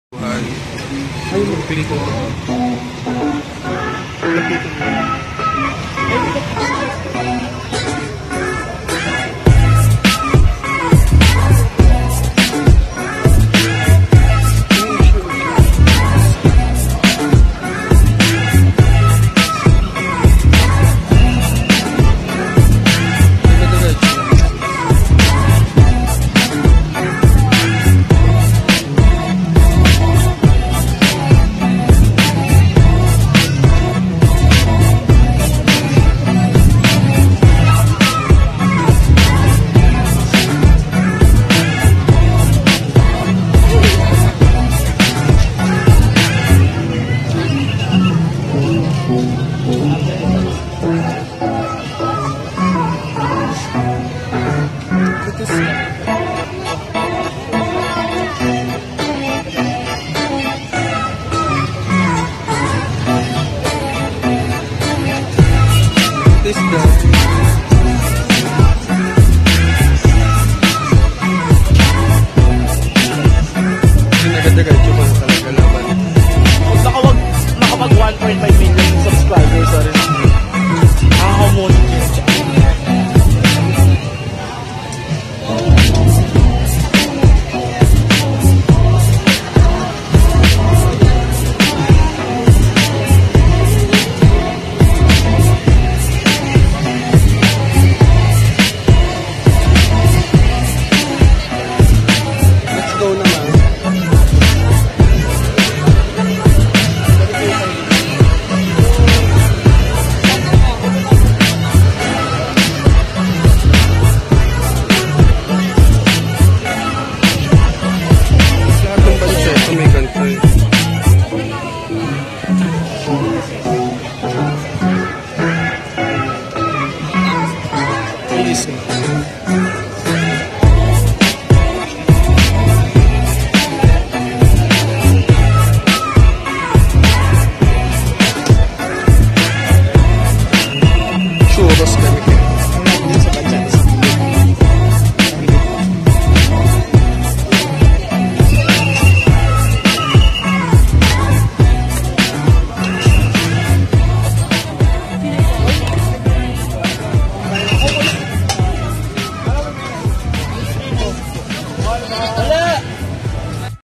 Banchetto Night at Robinsons Town Mall Malabon
A Banchetto Night Street Food Market at Robinsons Town Mall Malabon.
Located at:
Robinsons Town Mall Malabon Gov. Pascual Avenue, Brgy. Tinajeros, Malabon City.

Pls do like his channel too! For an awesome musics.